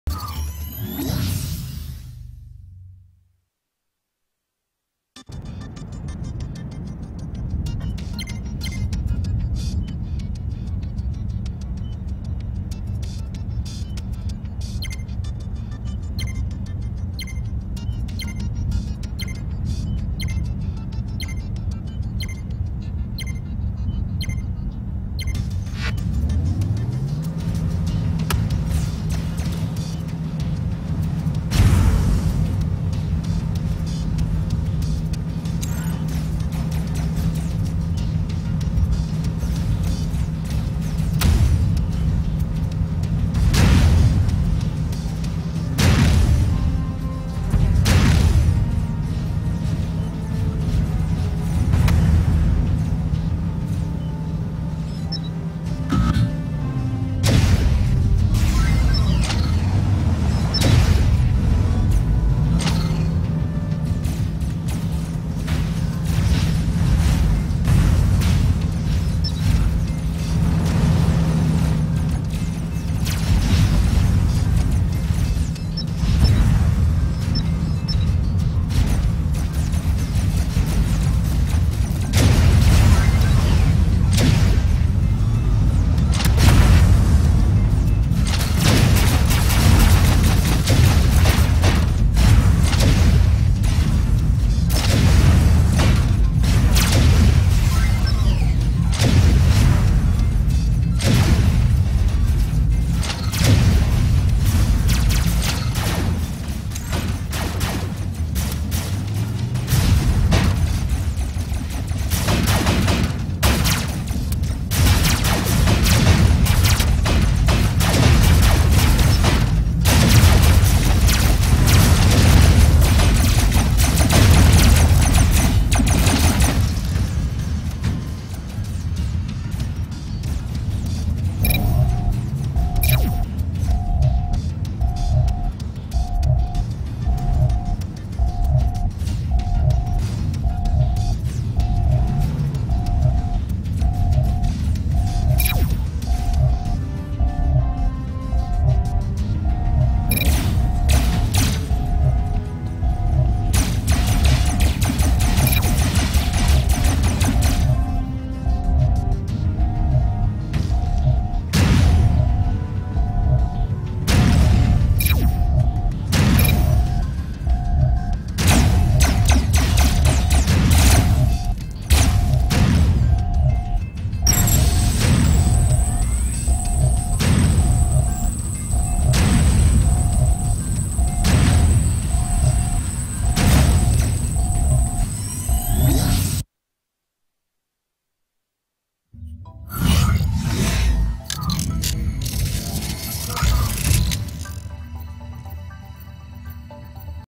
M23 Assault Bike Video A: Bike Assault!
The M23 Assault Bike managed to infiltrate the enemy's territory and pretty much destroys their team.